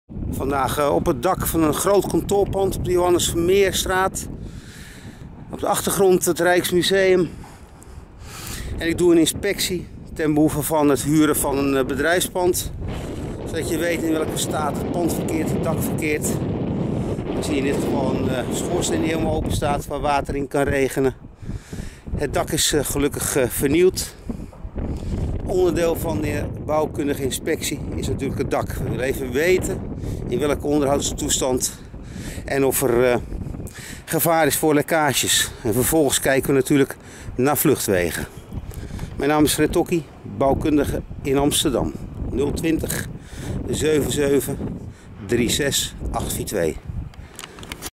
Bouwkundige keuring bedrijfspand Amsterdam Zuid
Bekijk de bouwkundige keuring van een bedrijfspand in Amsterdam Zuid. Leer alles over het kopen of het huren van een bedrijfspand in Amsterdam Zuid.